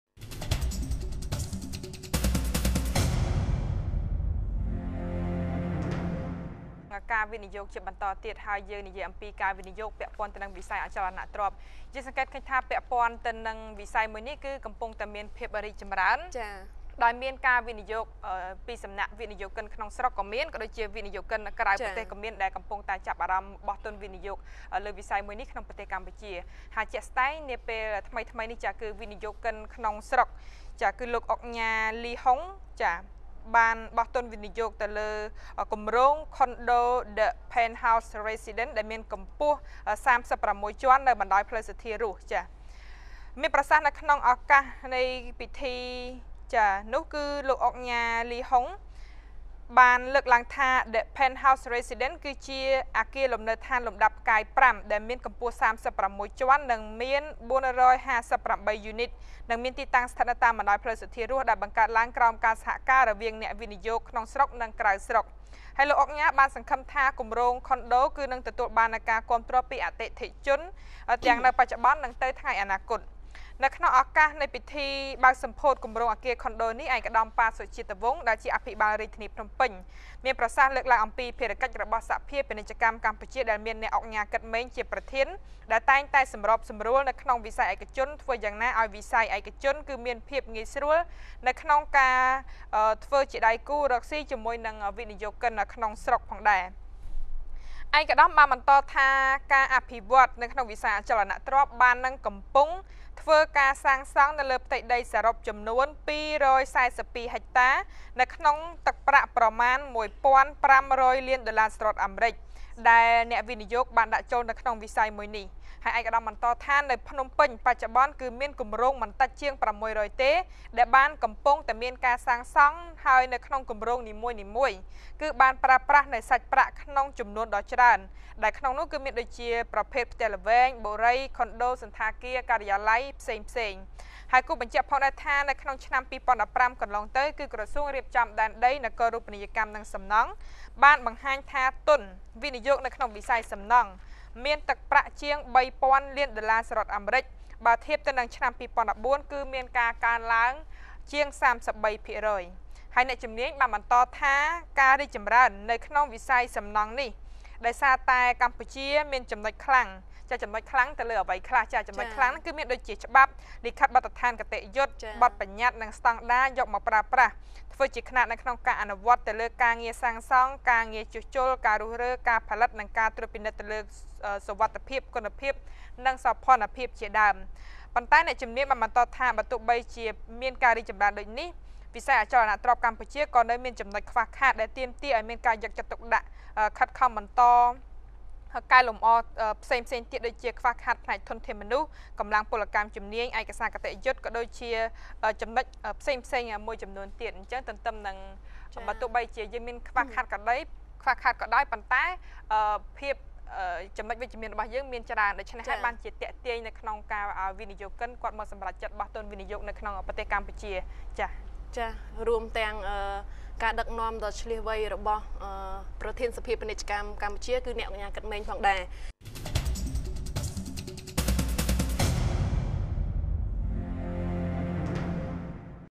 Grand Opening by CNC TV
Affordable luxury living space that match 5 stars international standard in the heart of Phnom Penh, Cambodia